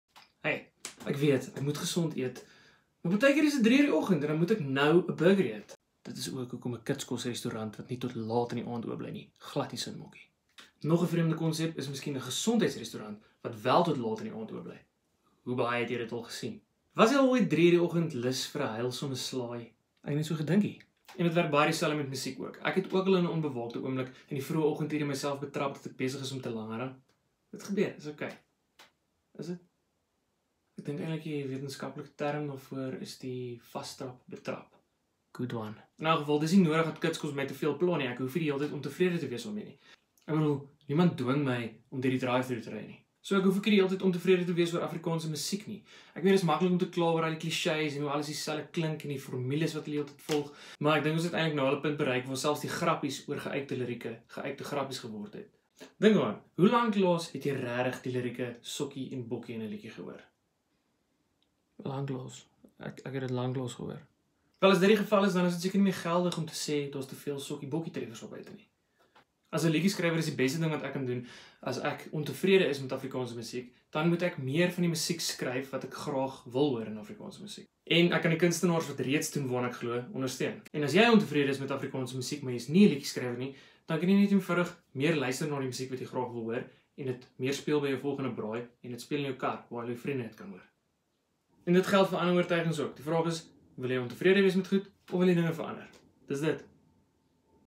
Deon Meiring - Hoe om te kla oor Afrikaanse musiek
Miskien het jy jouself ook al 03:00 die oggend gevang dat jy besig is om te langarm. Moenie te hard wees op jouself nie. Vraag is: Wil jy ontevrede wees of wil jy dinge verander?